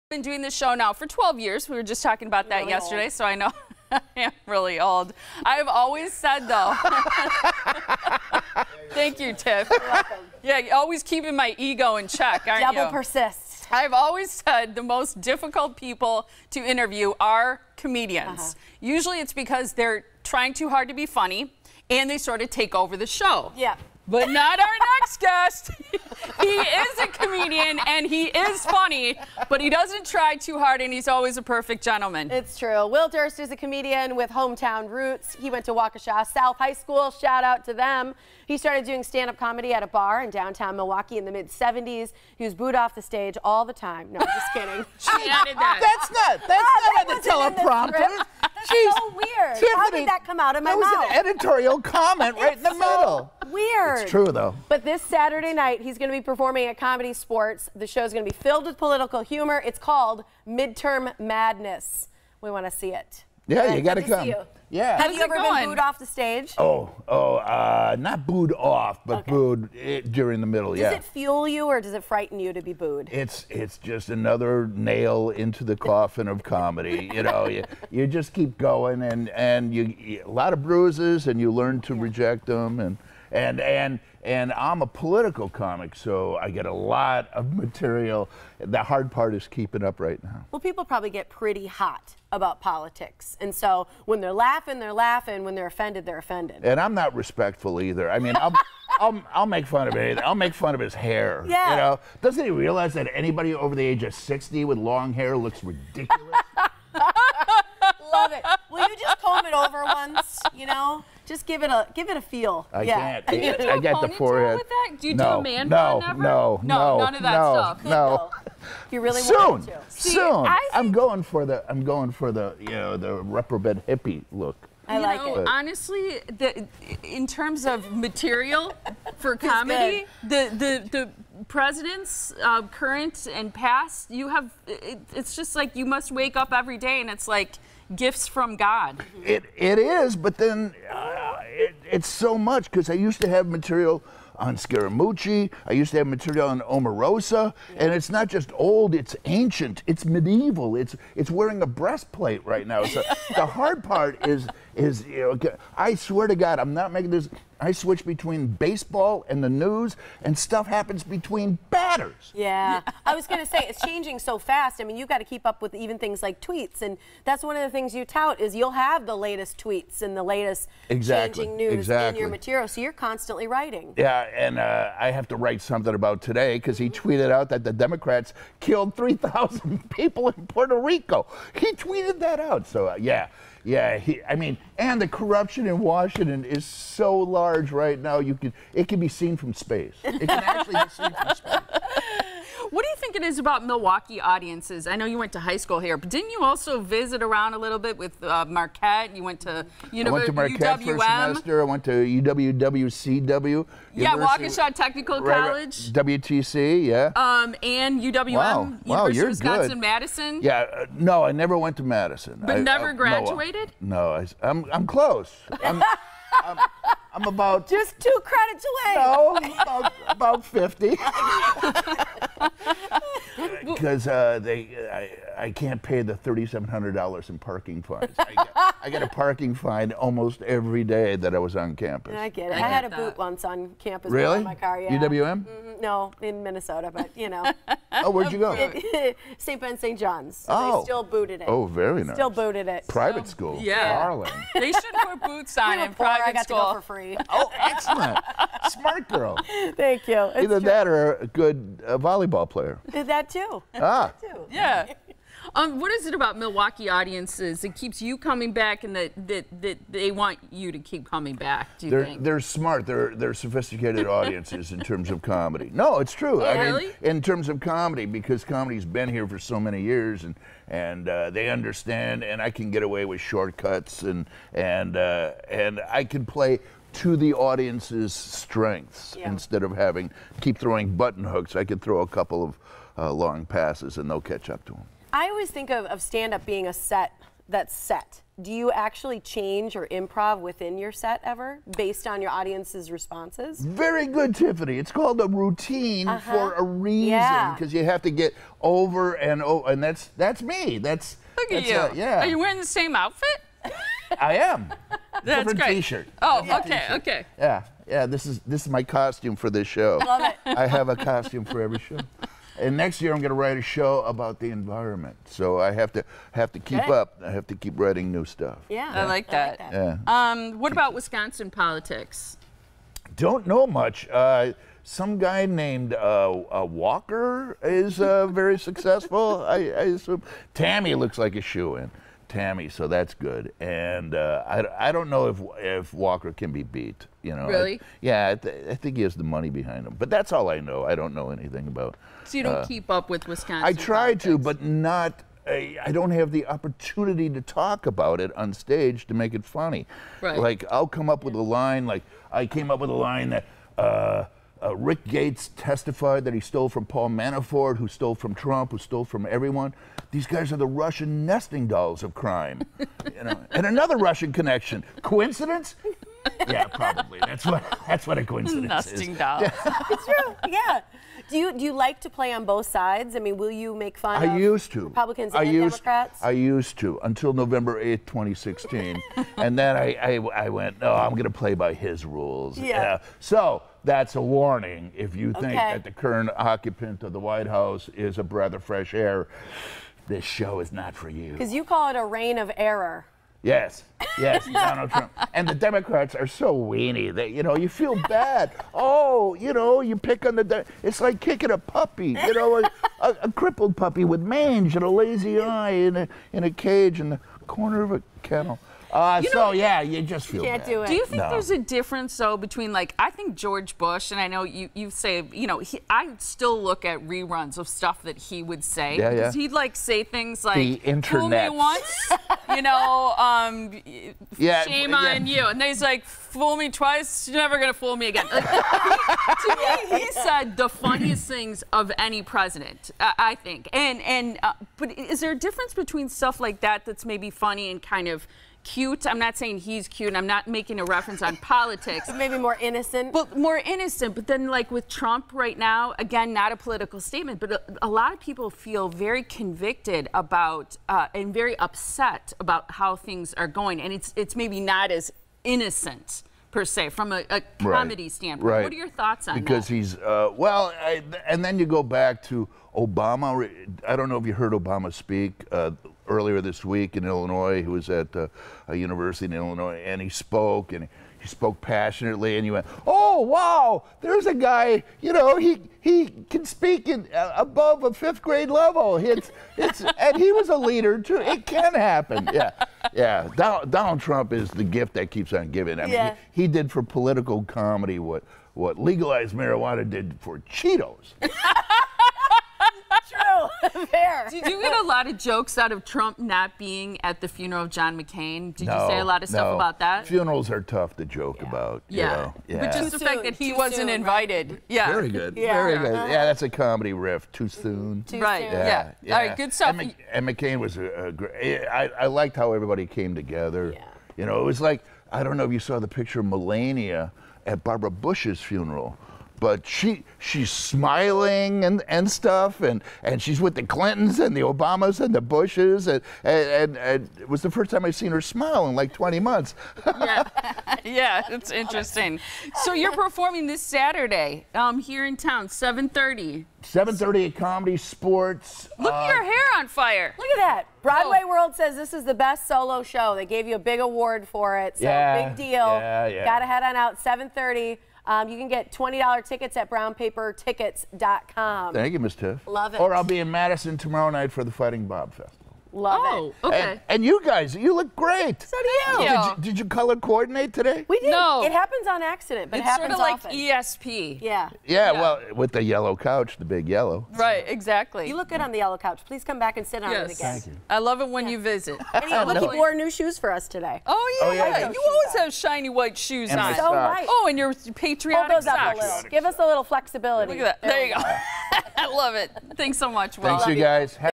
The Hilarious Will Durst Discusses the Midterms
As a political comedian in today's world, Will Durst always has A LOT of material to work with during his shows. Whenever he's in town, he's sure to make a stop on our Yellow Couch, and this time is no exception. Will joins us to discuss the midterms and his upcoming show in Milwaukee.  You can see Durst Case Scenario: Midterm Madness on Saturday, September 15 at Comedy Sportz. For more information and for tickets, visit BrownPaperTickets.com.